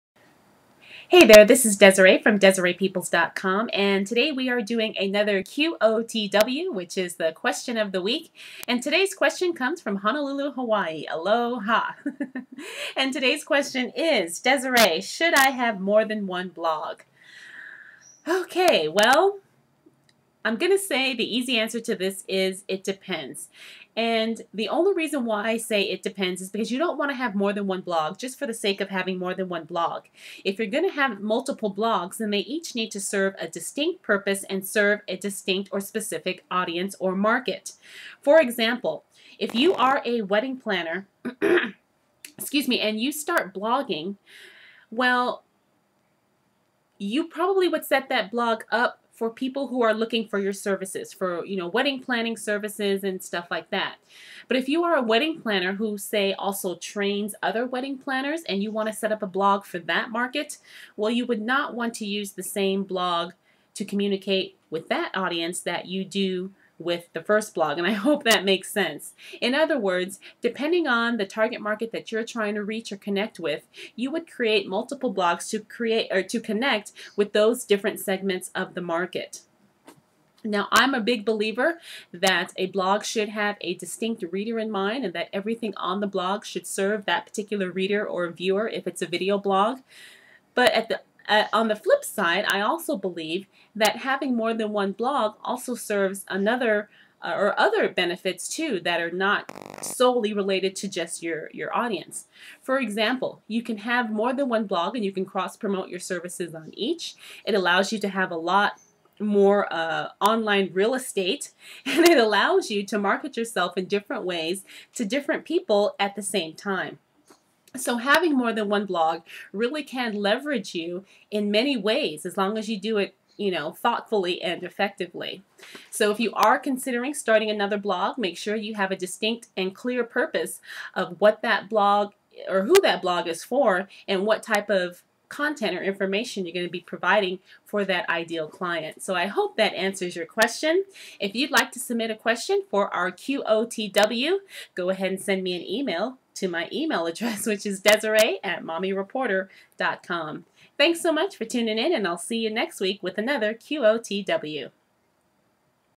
QOTW #3: Should you have more than one blog?
Today's Question of the Week is about whether or not you should have more than one blog...